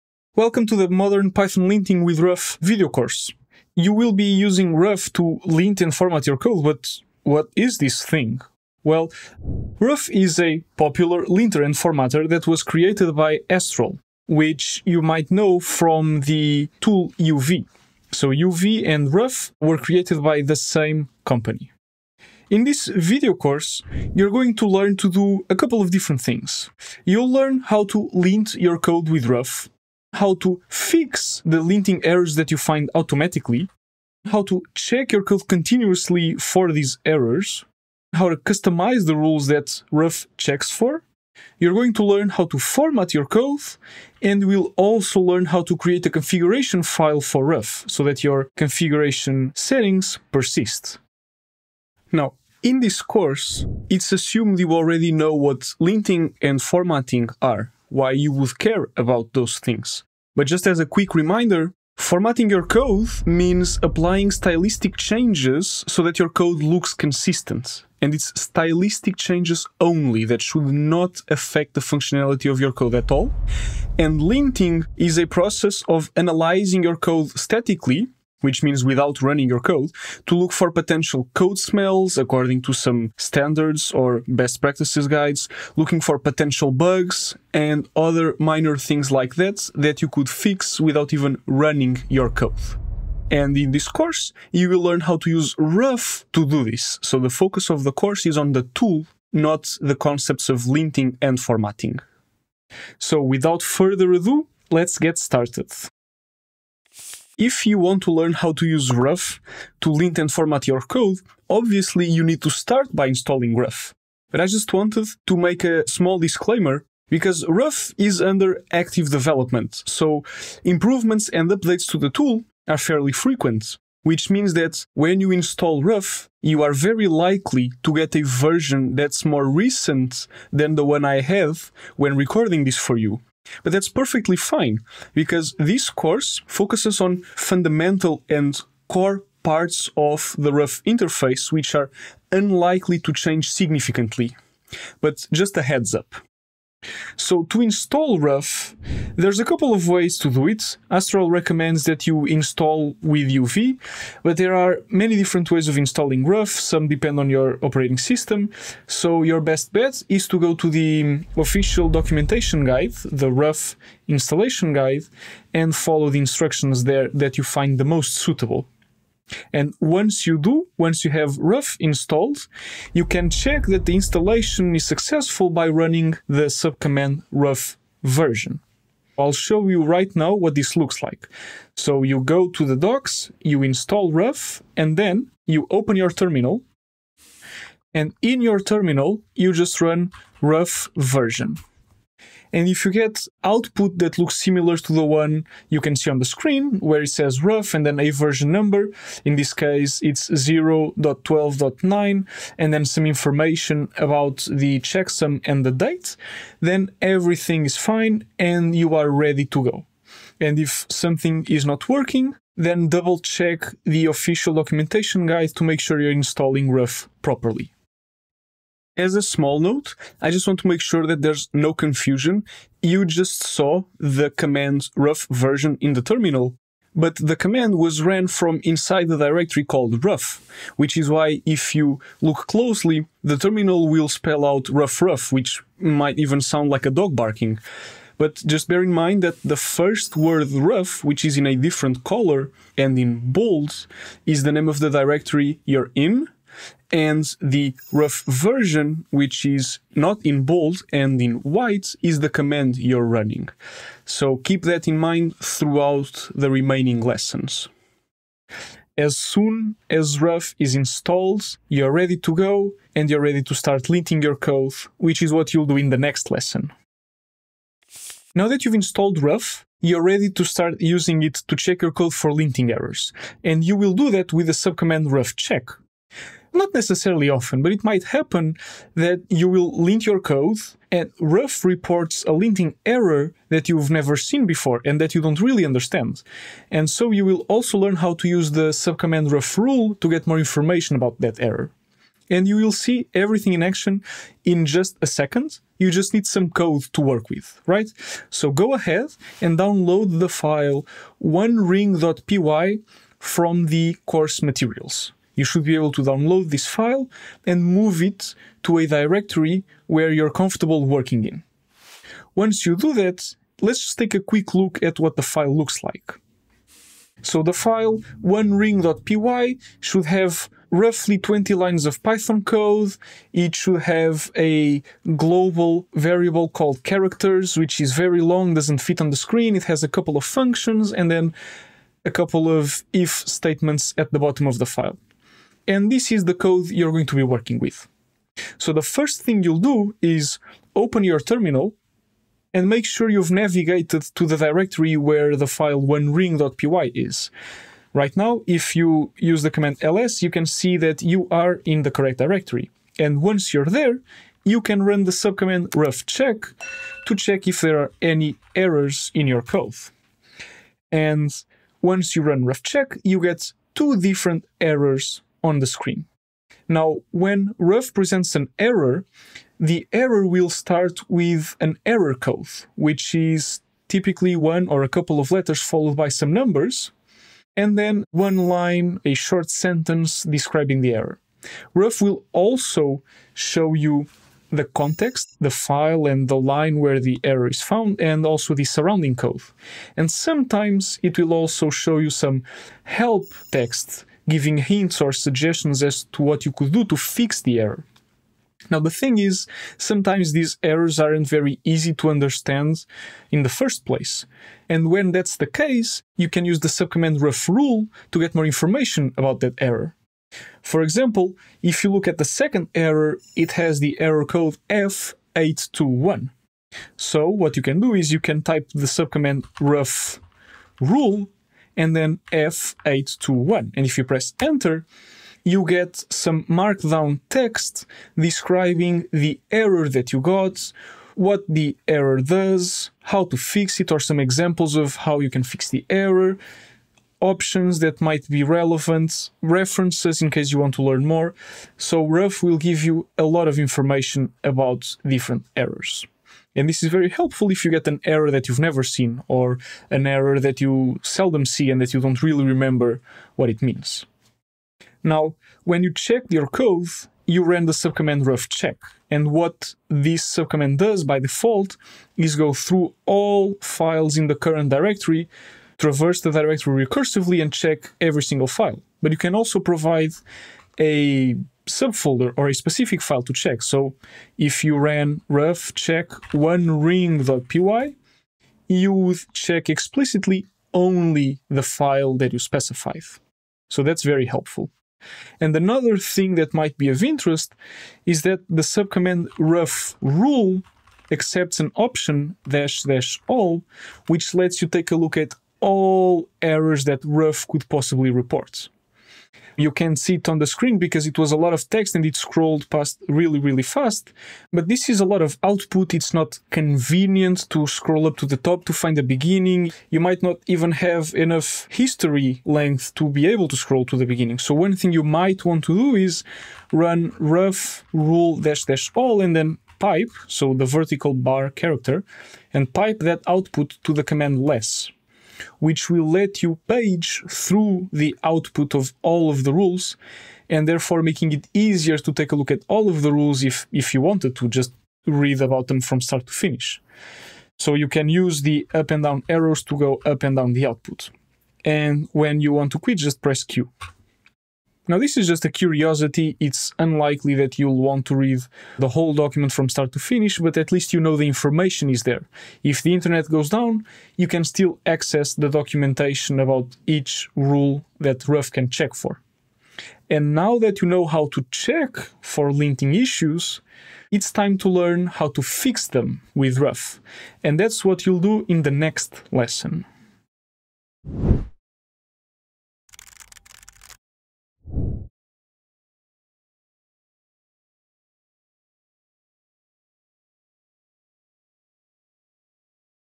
Modern Python Linting With Ruff: Installing Ruff & Linting Your Code
This is a preview of the video course, "Modern Python Linting With Ruff". Linting is essential to writing clean and readable code that you can share with others. A linter, like Ruff, is a tool that analyzes your code and looks for errors, stylistic issues, and suspicious constructs. Linting allows you to address issues and improve your code quality before you commit your code and share it with others.

Ruff is a modern linter that’s extremely fast and has a simple interface, making it straightforward to use. It also aims to be a drop-in replacement for many other linting and formatting tools, such as Flake8, isort, and Black. It’s quickly becoming one of the most popular Python linters.

The rest of the course covers:
- Fixing Linting Errors Automatically
- Checking Your Code Continuously
- Widening the Linting Scope
- Formatting Code With Ruff
- Configuring Ruff

👉 Ready to go deeper? Courses & membership